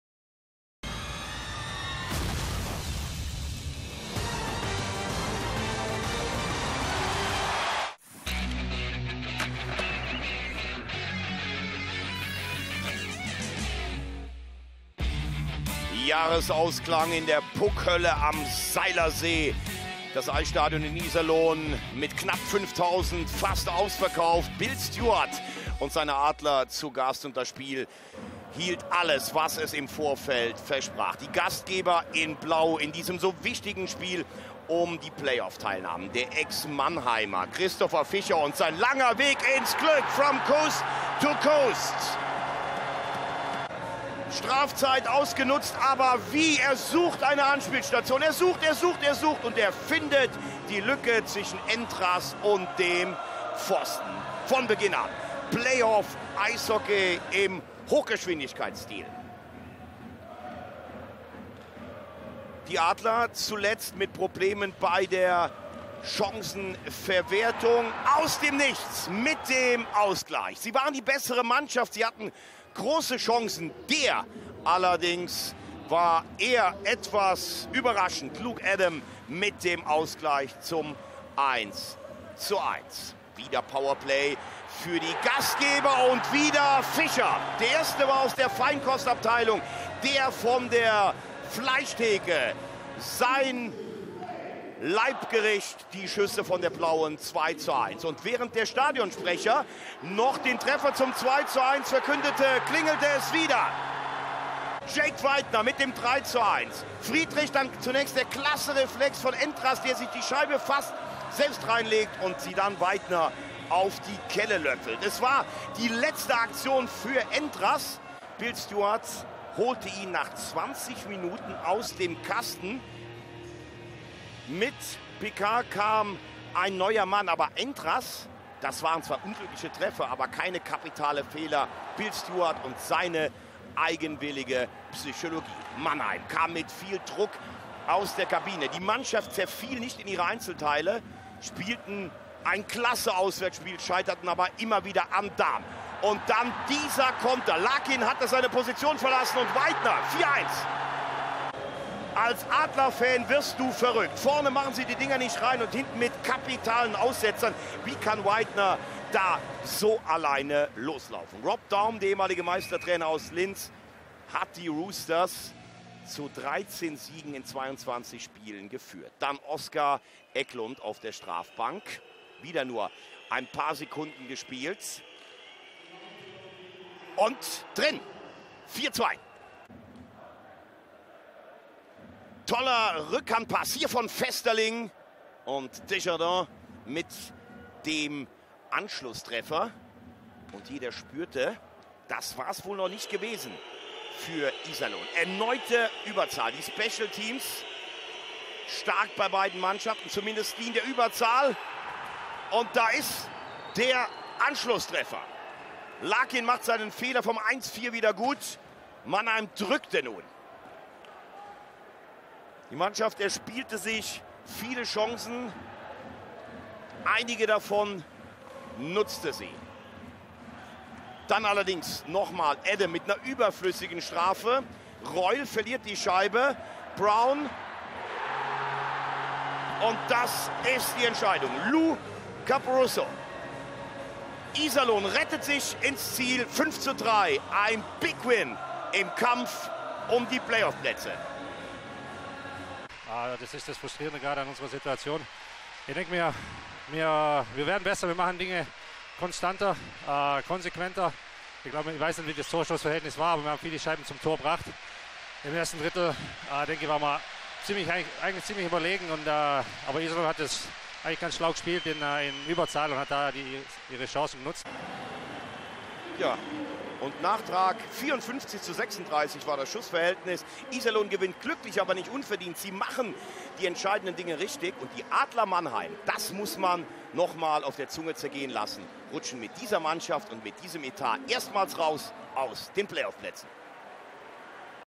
Iserlohn Roosters - Adler Mannheim | 36. Spieltag, 17/18 | Telekom Sport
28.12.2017, 36. Spieltag der Saison 2017/2018. Wieder keine Punkte für die Adler aus Mannheim!

Die ersten 30 Minuten müssten sich für die Roosters angefühlt haben wie ein Trainingsspiel. Kaum Gegenwehr und Präsenz der Gäste. Iserlohn ließ sich nicht zweimal bitten und so stand es vor dem letzten Drittel schon 4:1 für den Gastgeber.
Zwar kamen die Adler dann mit einem Doppelschlag wieder auf 4:3 heran, aber Iserlohn hatte auch darauf eine Antwort und nutzte ein Powerplay zum 5:3 Schlusspunkt.

Aufstellungen:

Adler Mannheim:
Andrew Desjardins, Matthias Plachta, Devin Setoguchi, Sinan Akdag, Mathieu Carle, Dennis Endras

Iserlohn Roosters:
Jake Weidner, Marko Friedrich, Justin Florek, Dieter Orendorz, Johan Larsson, Sebastian Dahm